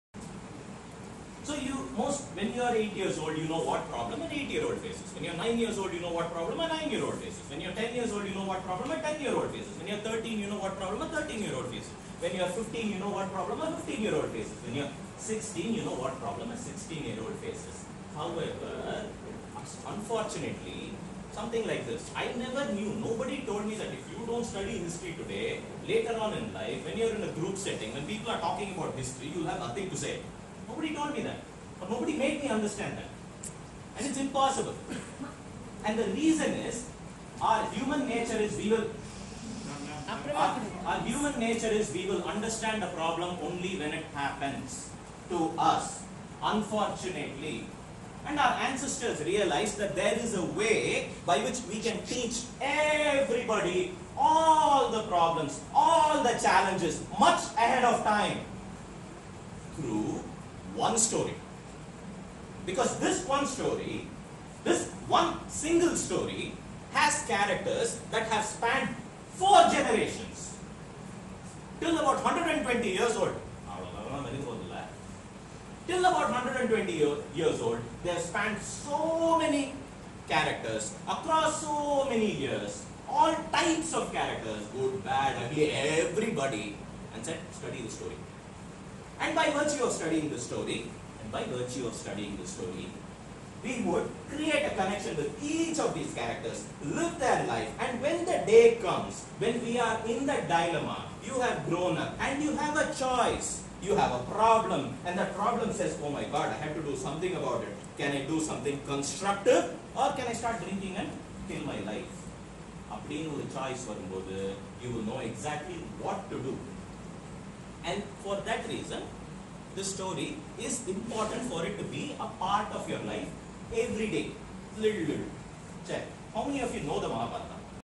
Mahabharata is a Life Teacher - Sriram Raghavan
The Mahabharata spans stories across 4 generations and thus acts as a life teacher, helping us understand tough situations even before we face them.